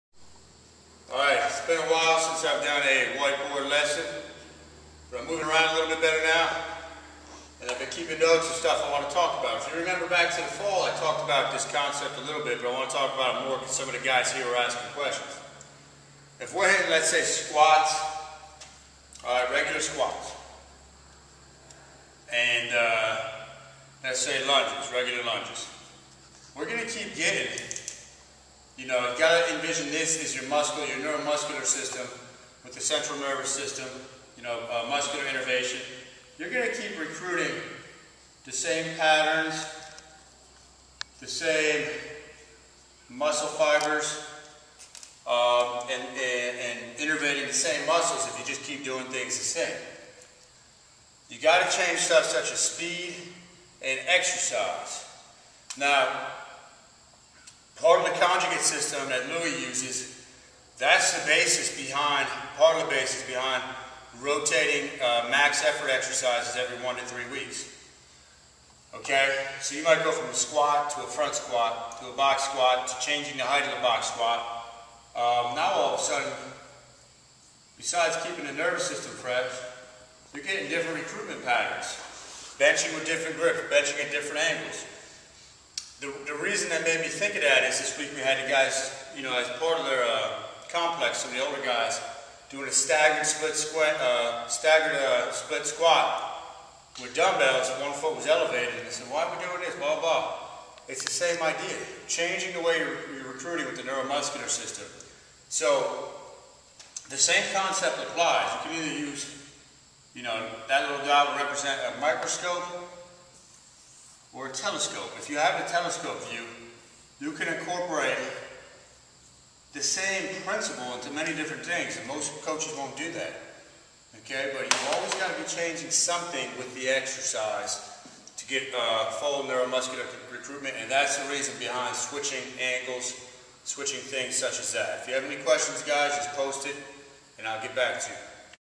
How to Create Different Neural Pathways in the Muscles
If you pour a cup of water from the same position onto a sand hill, you will continually get the save grooves.  This is like doing the same exercise over and over and the muscles will only get recruited from in one way.  Gotta change it up...